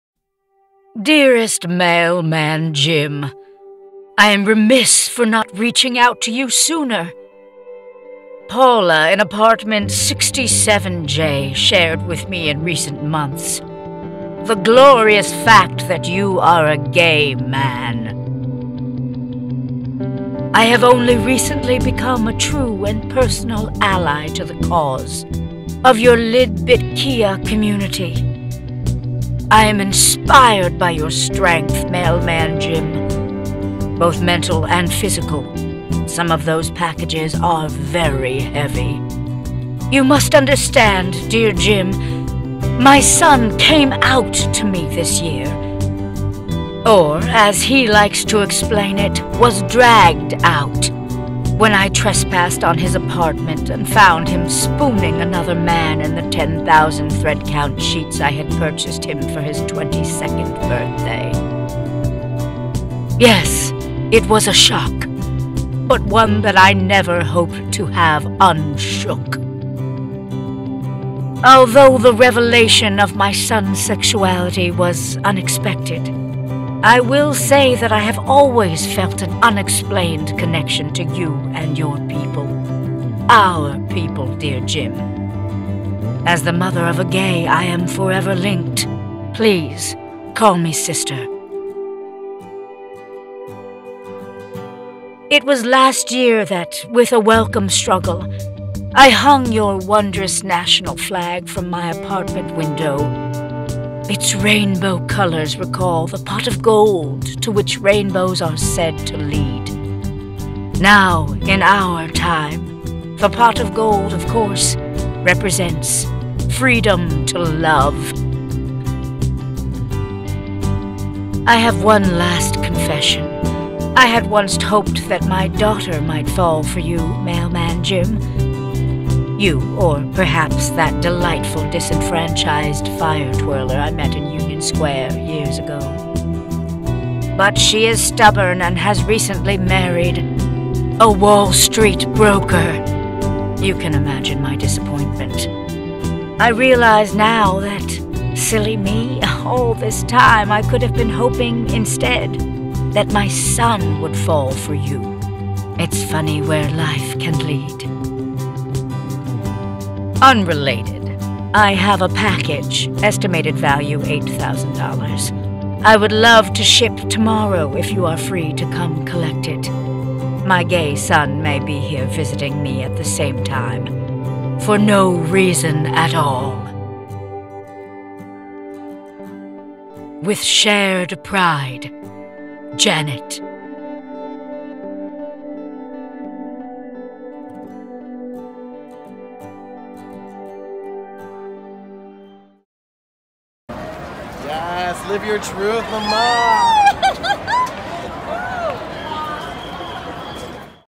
Letters From Janet - PRIDE
Janet is a wealthy, over-dramatic, well-meaning but misguided Manhattanite who FEELS YOUR PAIN.  In this letter, she writes to her mailman Jim in celebration of Gay Pride.

Written, performed & edited by Liz Days
Filmed during the 2019 NYC Pride March by Laura Daniel Koch